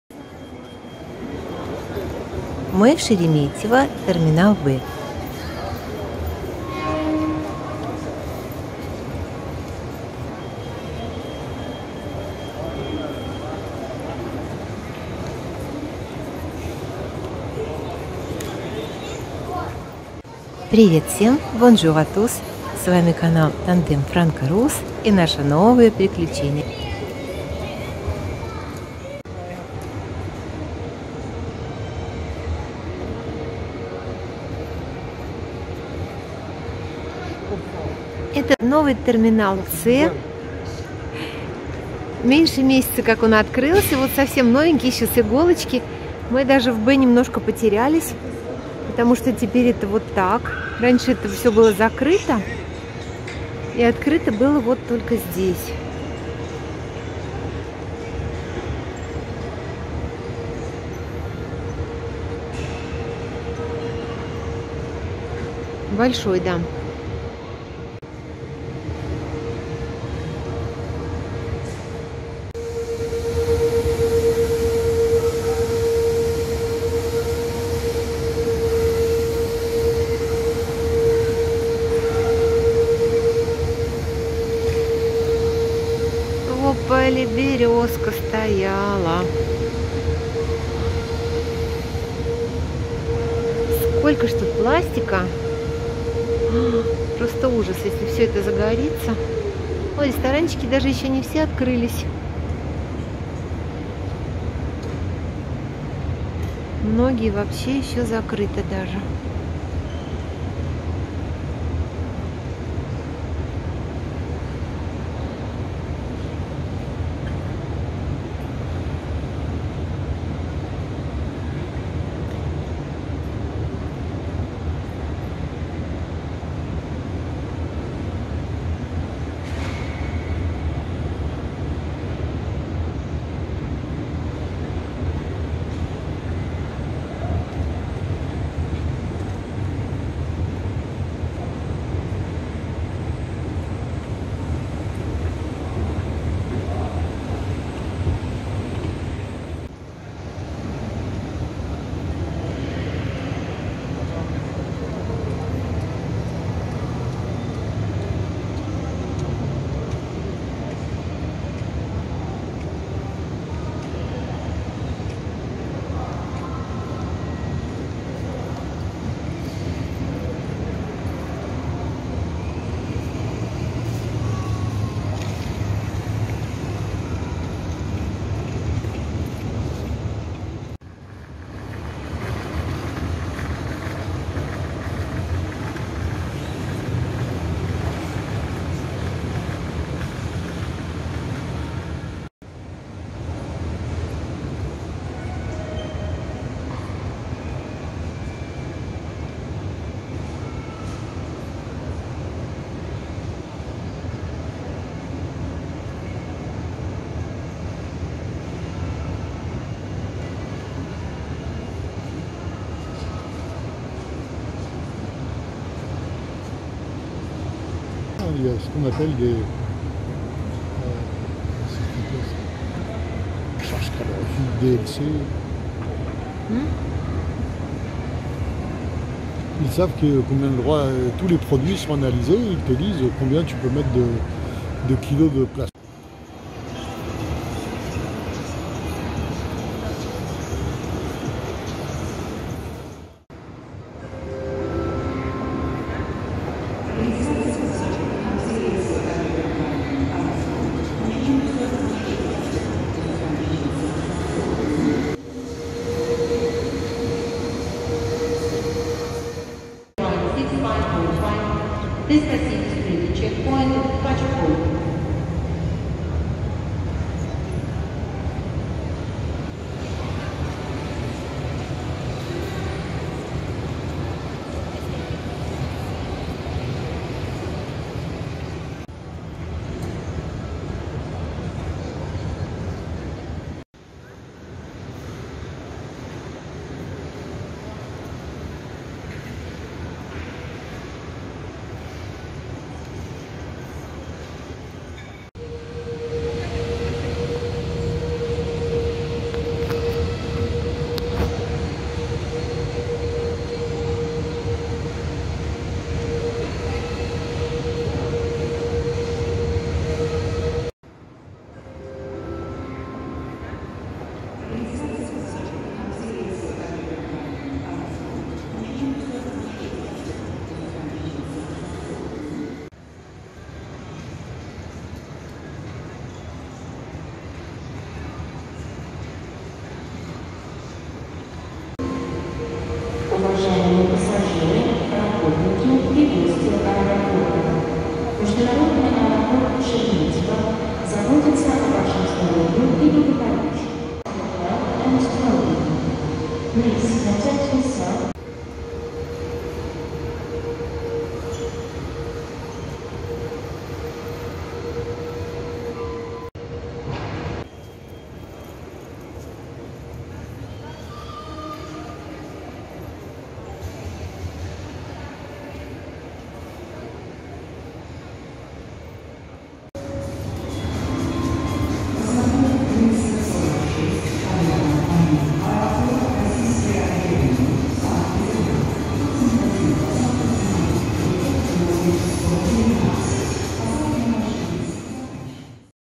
Аэропорт Шереметьево. Новый терминал С
Как и обещали в предыдущем видео, приглашаем на прогулку по новому терминалу С в аэропорт Шереметьево , оно того стоит - там и правда очень красиво. Самый современный и высокотехнологичный в России, просторный, стильный.

Путешествуйте с нами !